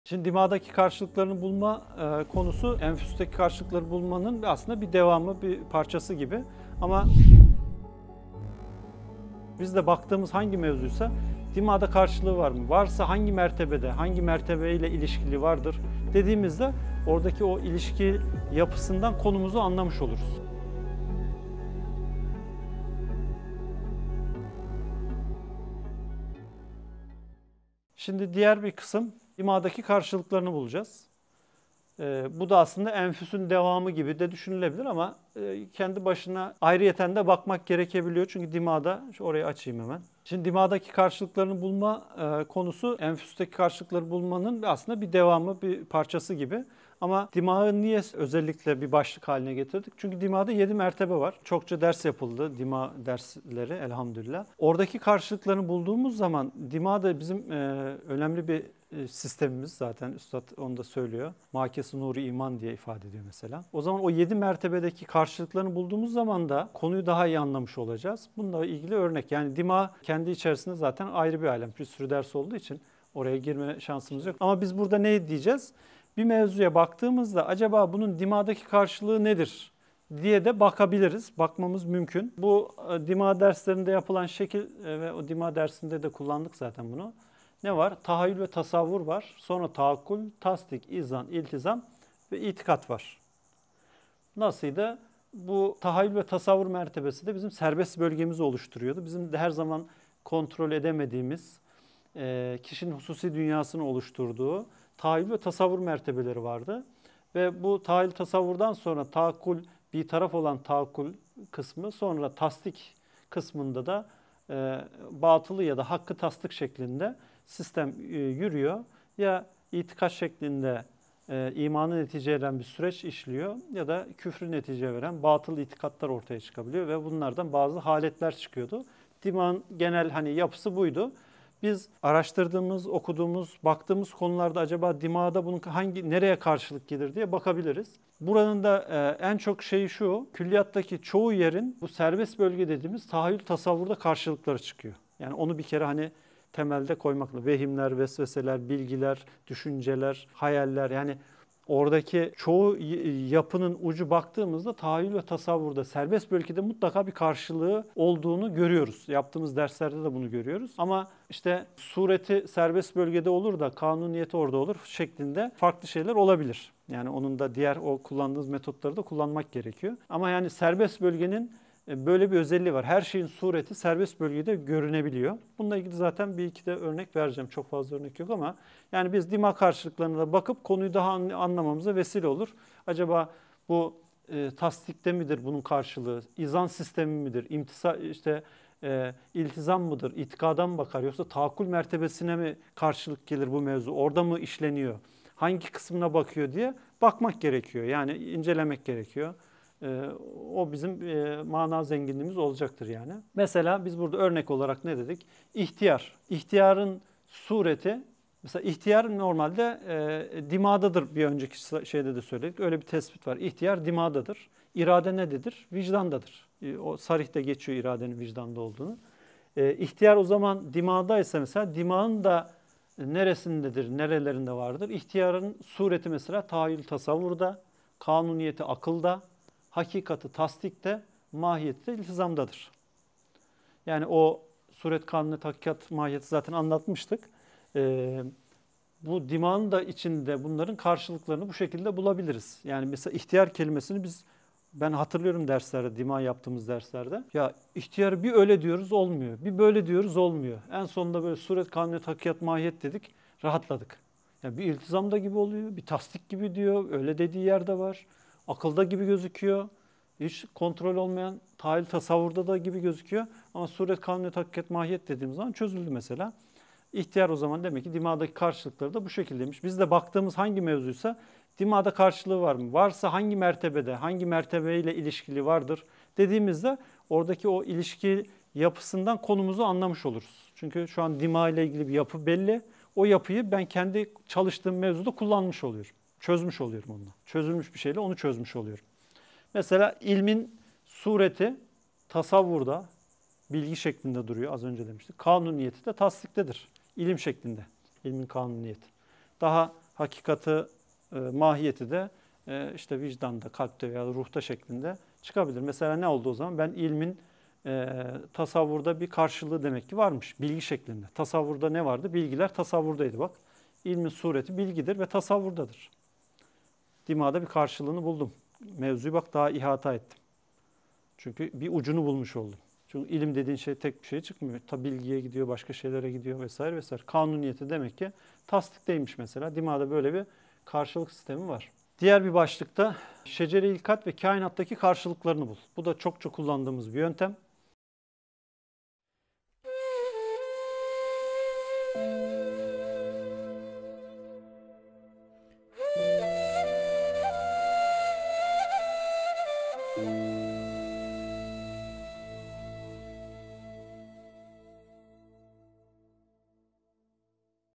DİMAĞDAKİ KARŞILIKLARINI BUL | B:18
Selamun Aleyküm! Uzun zamandır beklenen ve çok sorulan bir meseleyi güzel bir çalışma hazırlamaya muvaffak olduk. RİSALE-İ NUR'DA ÇALIŞMA YÖNTEMLERİ başlığı ile başladığımız seriyi inşaAllah sizlerin istifade ve istifazalarınıza sunamaya devam ediyoruz,

18.bölüm

SORULARINIZI VE ÇALIŞMALARINIZI BİZİMLE YORUMLAR KISMINDA VEYAHUT İNSTAGRAM MESAJLAR KISMINDAN ULAŞTIRABİLİRSİNİZ.